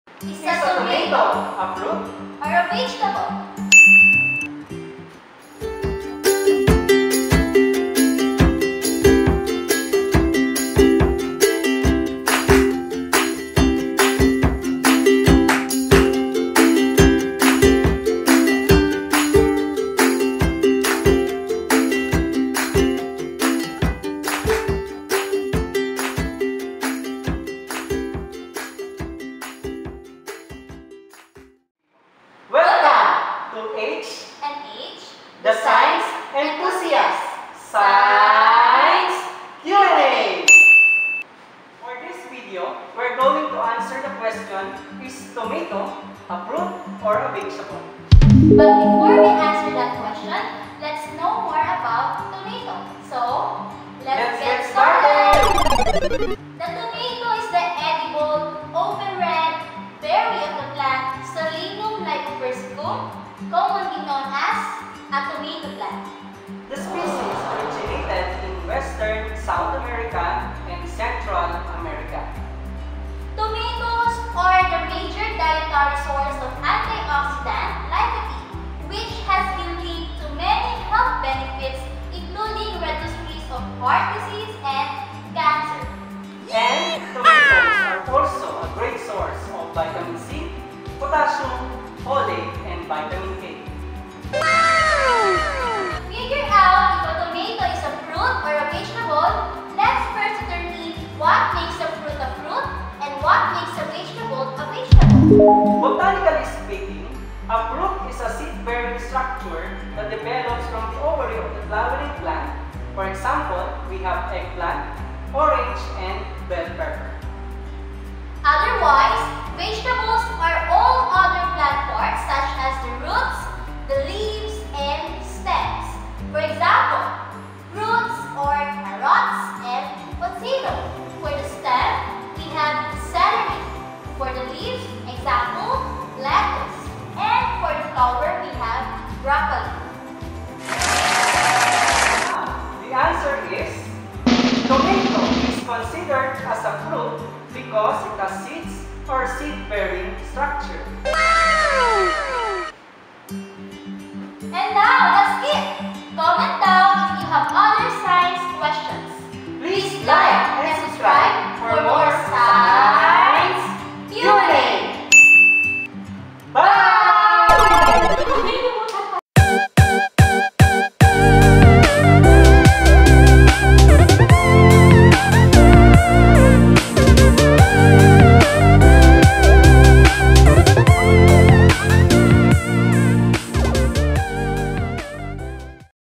H&H: The Science Enthusiasts - Is a TOMATO a fruit or a vegetable? || Science Question & Answer
Is a TOMATO a fruit or a vegetable? 🍅🍅🍅

To really figure out if a tomato is a fruit or vegetable, you need to know what makes a fruit a fruit, and a vegetable a vegetable. The big question to ask is, DOES IT HAVE SEEDS? If the answer is yes, then technically, (botanically) you have a FRUIT. This, of course, makes your tomato a fruit. It also makes cucumbers, squash, green beans and pumpkins all fruits as well. Along with the fruit from a plant or tree, we can often eat the leaves (lettuce,) stems (celery,) roots (carrots,) and flowers (broccoli.) Many of these other parts of the plant are typically referred to as VEGETABLES 🍉🍊🥦🥬🥔🧅🍇🌿🌳🎋

We would want to acknowledge the creators of ScienceBOB.com, our reference for this experiment. And the administrators of St. Martin Montessori School, Inc. for the venue of our shoot.